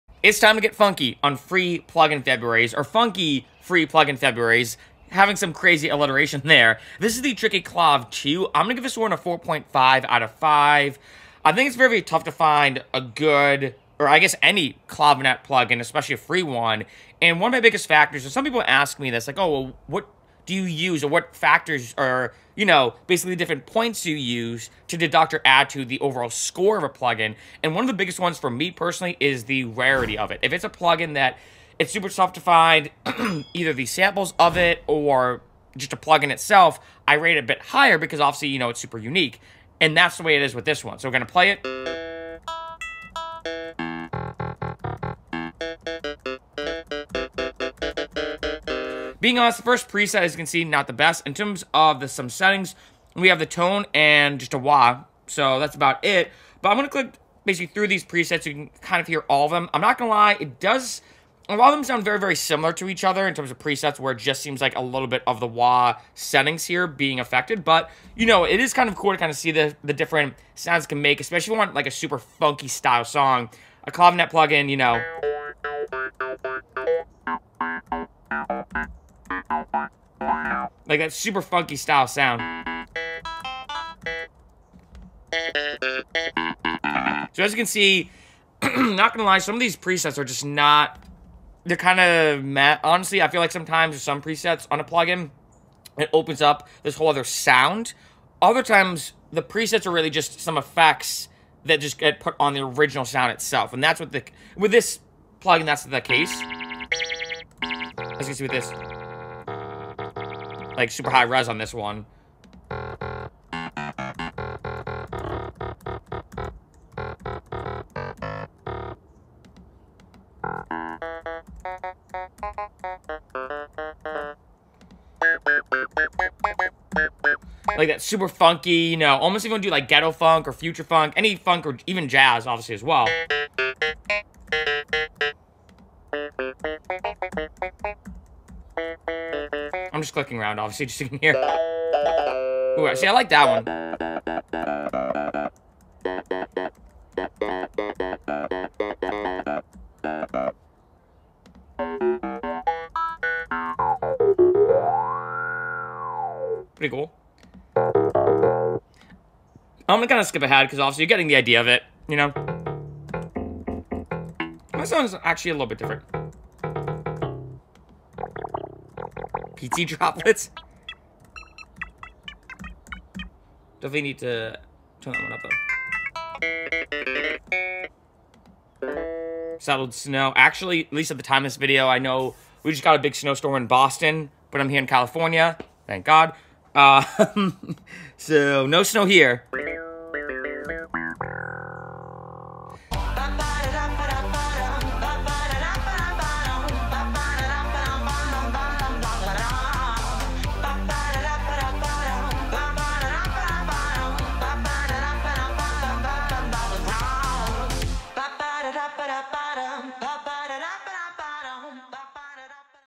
Ticky Clav 2 - FREE Clavinet VST Plugin
Ticky Clav 2, made by 2getheraudio, is a free clavinet VST plugin. Perfect for making that Clavinet Funk sound

FRECCERO RATING: 4.5/5